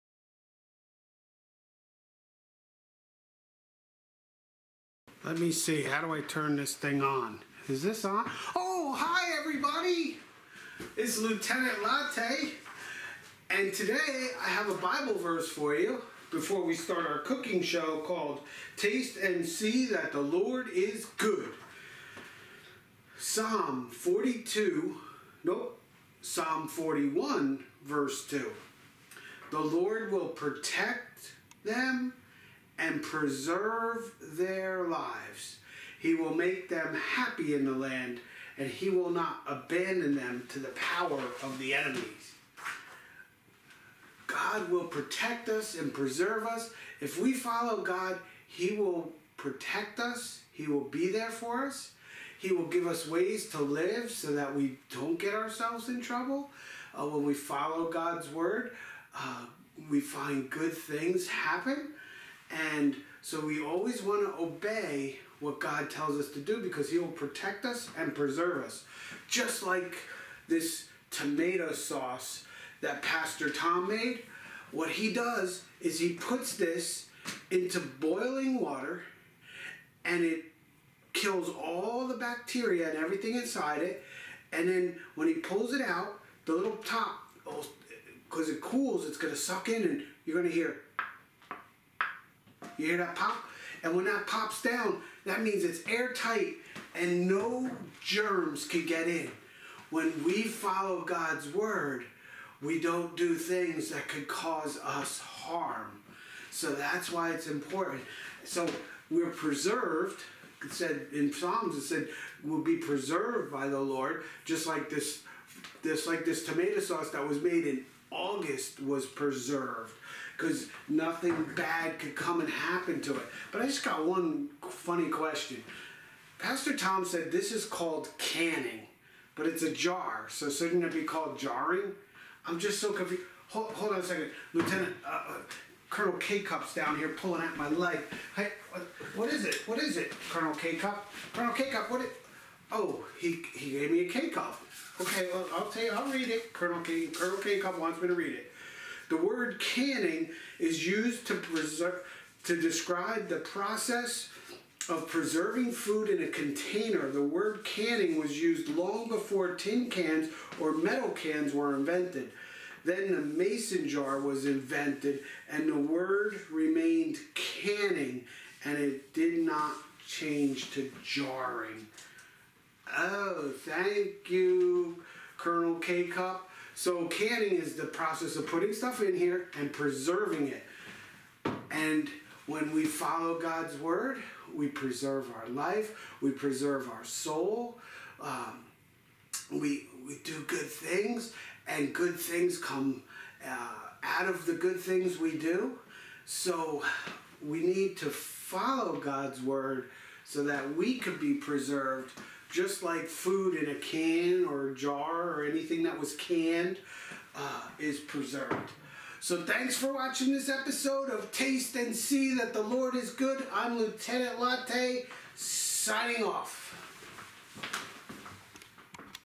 Taste and See that God is Good | Children's Message | Lieutenant Latte | 2/5/23
The second episode of Lieutenant Latte's new cooking show: Taste and See that God is Good.